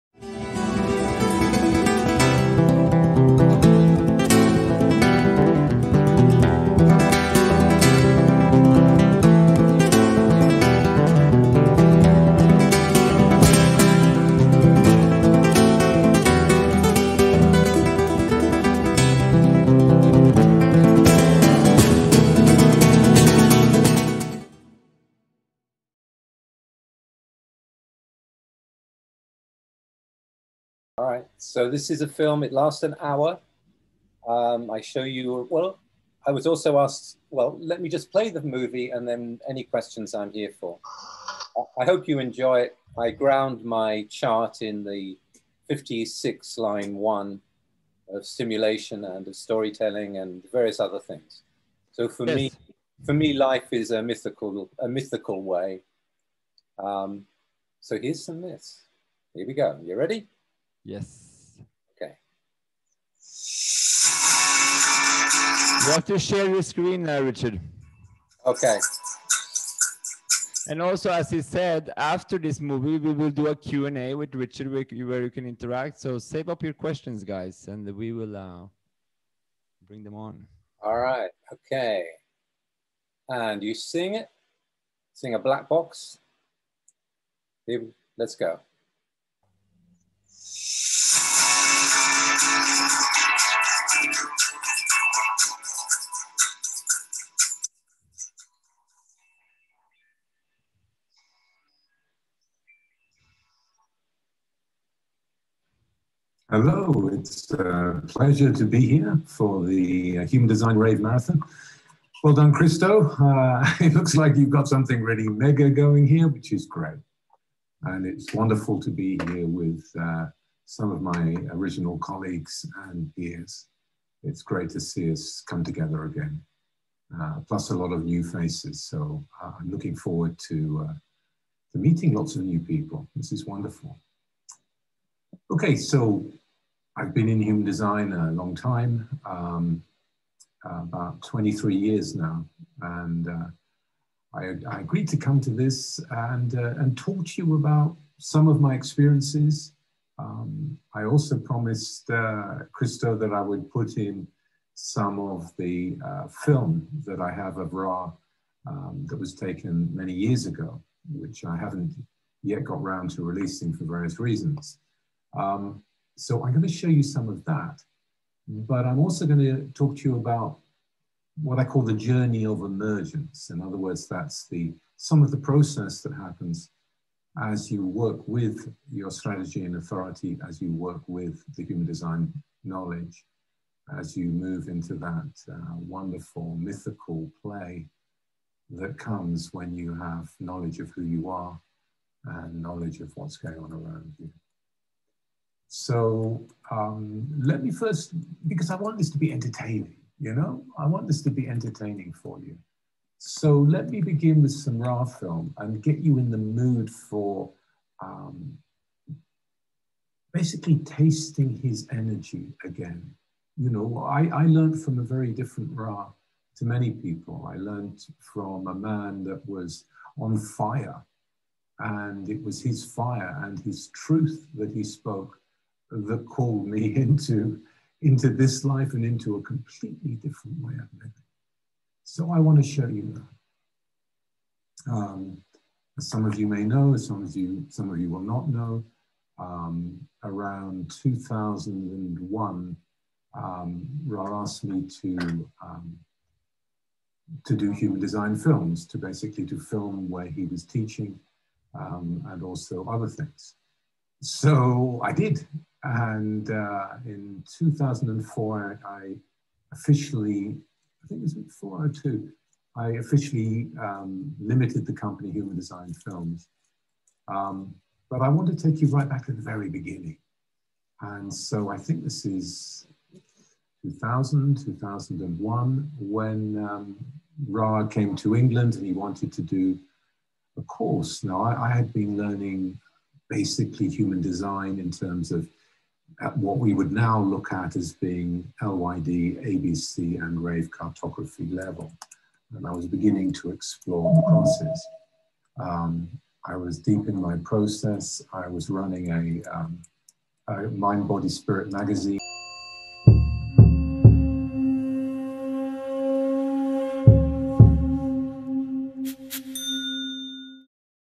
Human Design Rave Marathon 2021 - Richard Beaumont the Journey of Emergence
Human Design Rave Marathon 2021 - Richard Beaumont the Journey of Emergence (trailer version)